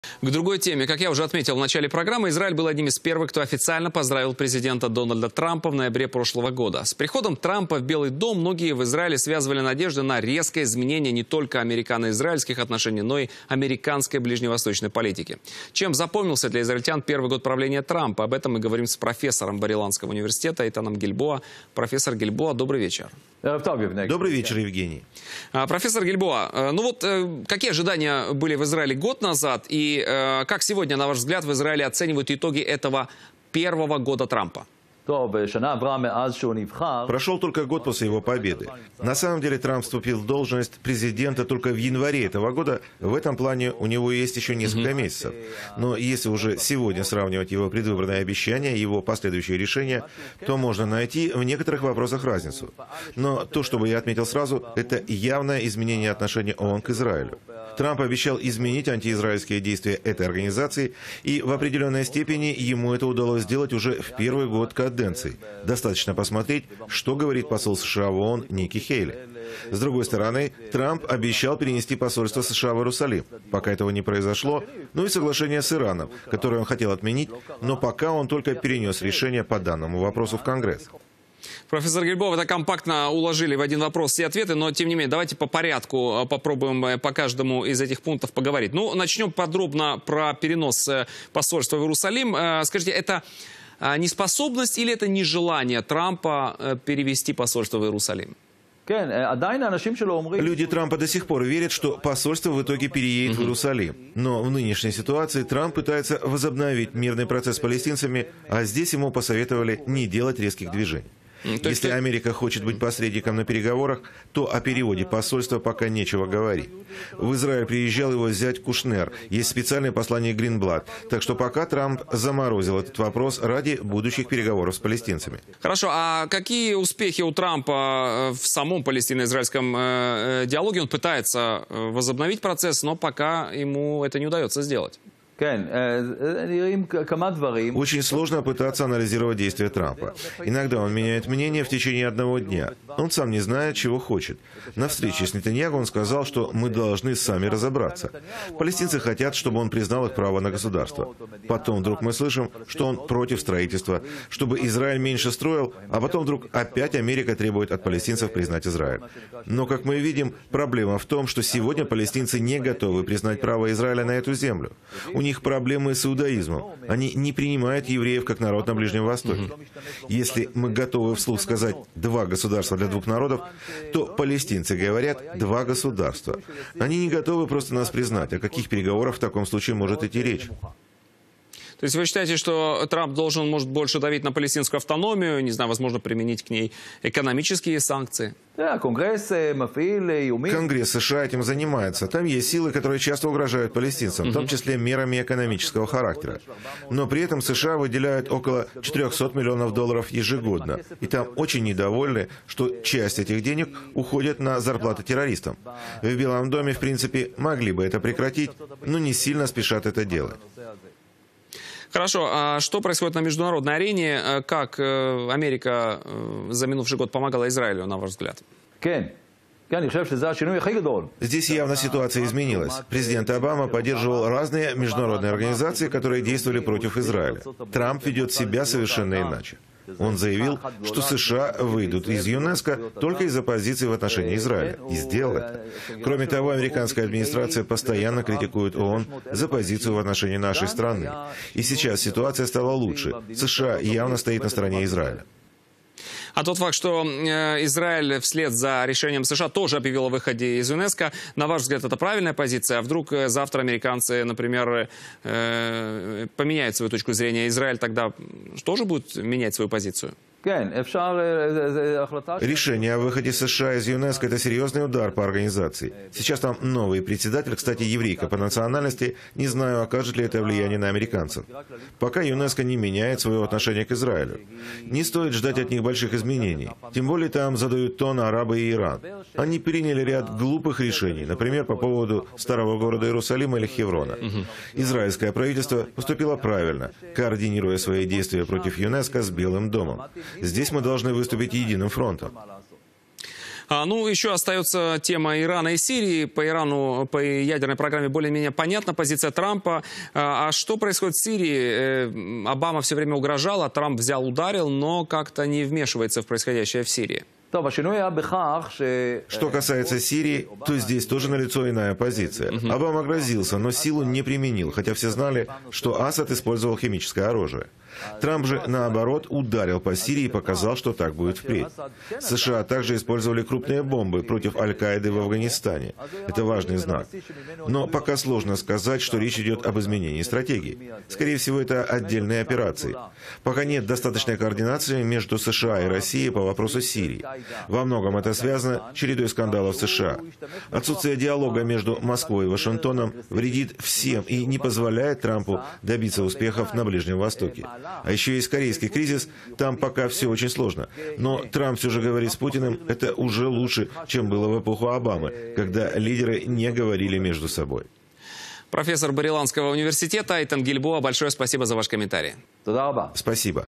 Первый год Дональда Трампа у власти в США. Каким его видят в Израиле?
Трамп сказал и не сделал! Как в Израиле оценивают итоги первого года работы американского президента и почему посольство США до сих не переехало из Тель-Авива в Иерусалим. Комментарий профессора Бар-Иланского университета Эйтана Гильбоа.